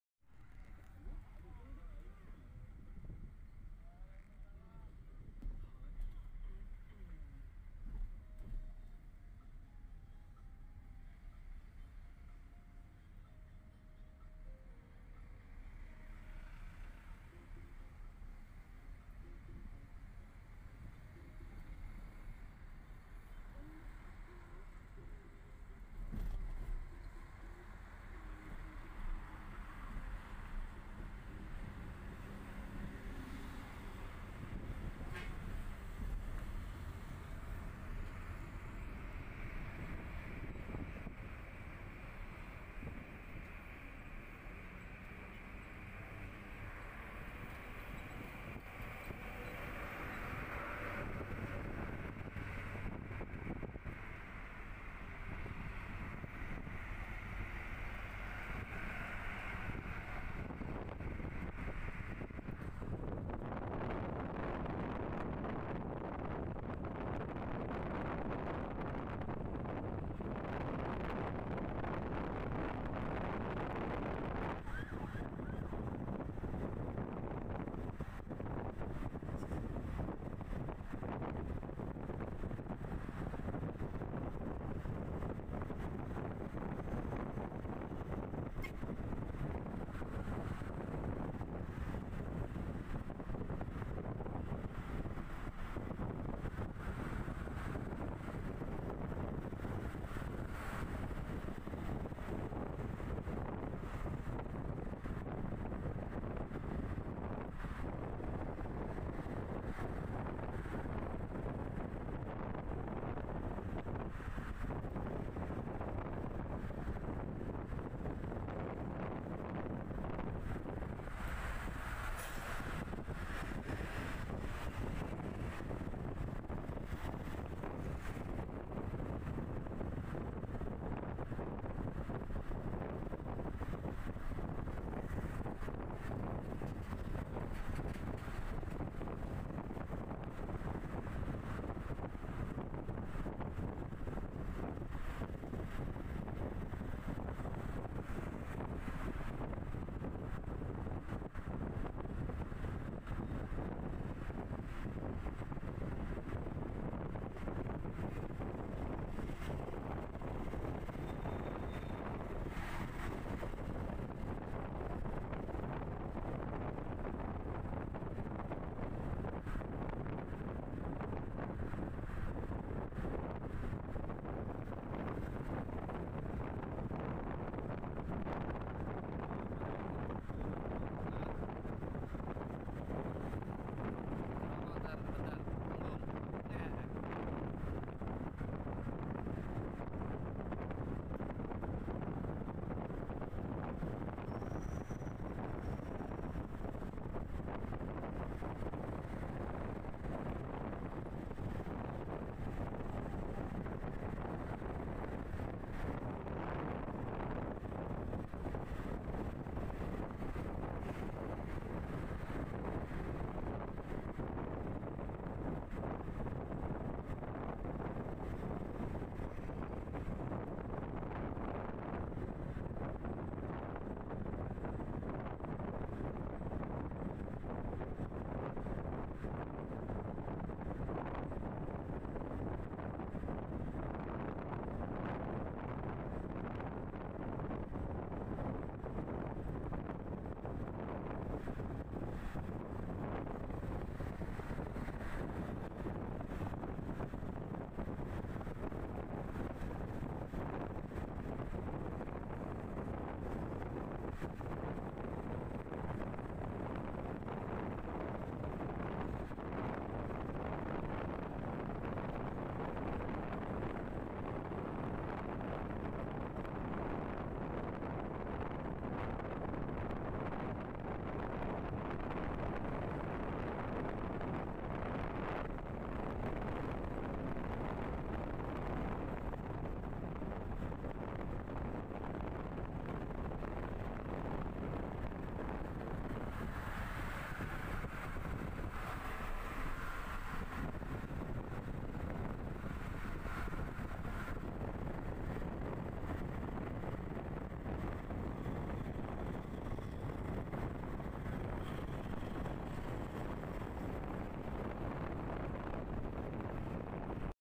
lihat pinggiran jalan Tol Jagorawi dari Jendela mobil ~ Lewat cibubur
perjalanan menuju jakrta dari kota Bogor ,melewati tol Jagorawi melihat pemandangan di tepian Jalan tol dari jendela kaca mobil
tol Jagorawi lihat pinggiran tol Jagorawi, dari jendela mobil,Cibubur.